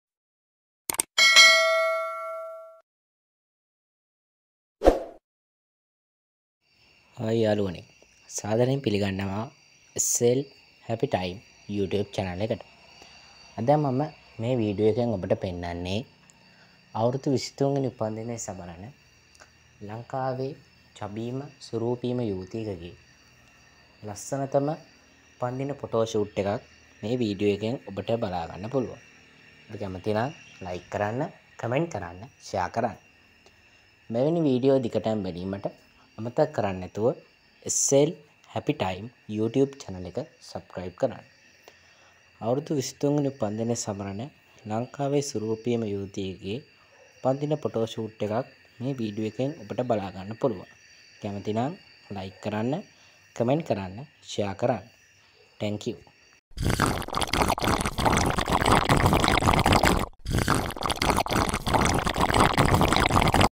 අවුරුදු 23වෙනි උපන්දිනය සමරන ලංකාවේ චබීම සූරූපී යුවතියකගේ ලස්සන පොටෝ shoot එකක් happy brith day
අවුරුදු 23වෙනි උපන්දිනය සමරන ලංකාවේ චබීම සූරූපී යුවතියකගේ උපන්දින පොටෝ shoot එකක්

මෙම පොටෝ වල අයිතිය මුල් අයිතිකරුවා සතුය